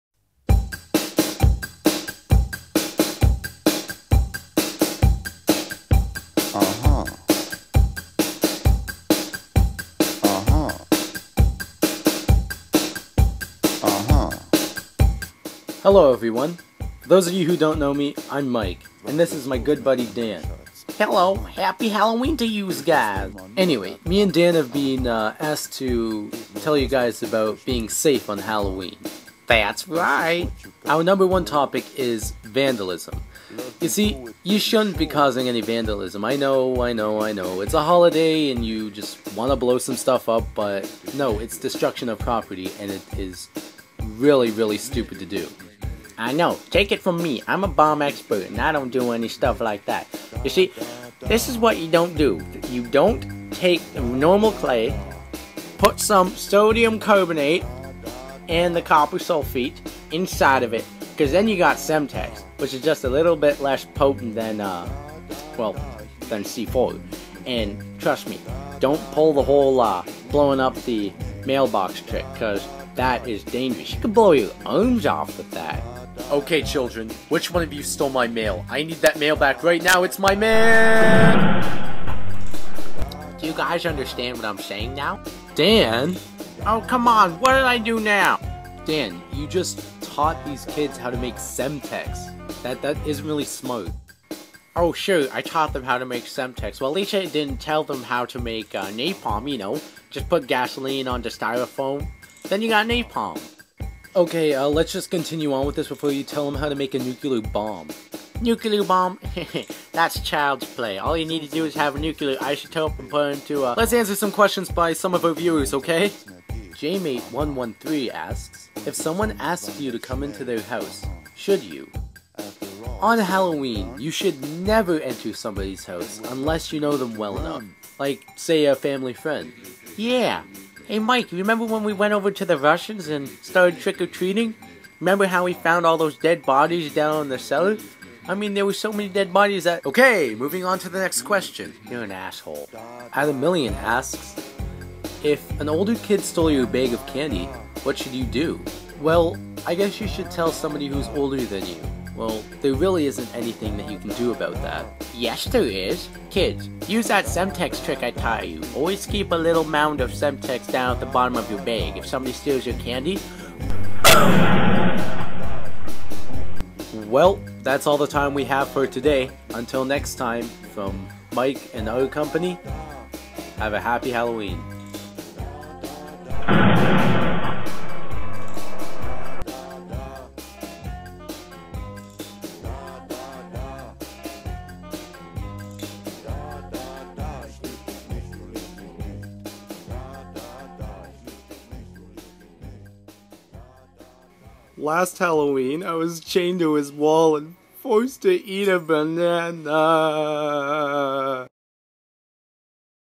Mike in real Life- Halloween Safety
Mike and Dan try to teach all you guys about Halloween safety while teaching you about SEMTEX and Napalm.